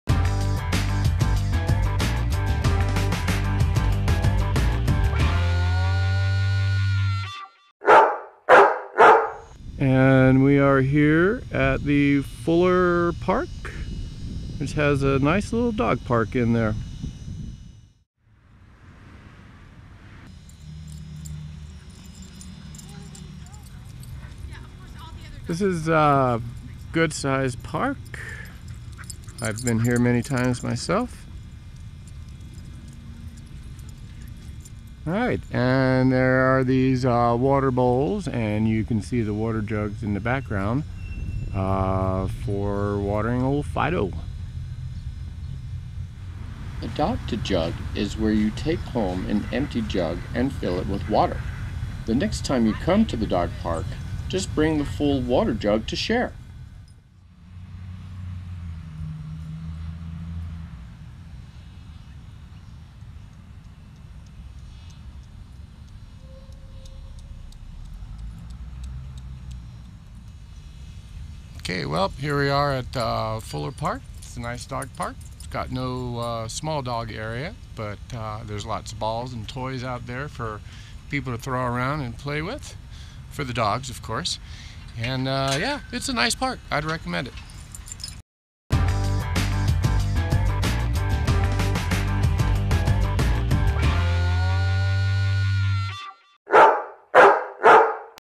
Dog Park at Fuller Park in Denver
Welcome to DENVER WAGS. Review for Fuller Park. The park is in Denver, CO. Located at 29th Avenue and Franklin Street. It is a nice size dog park. There is no area for small dogs. You should bring your own dog water or you can work with the park's Adopt a Jug where you take home an empty jug. Then when you come back next time just bring the full water jug to share.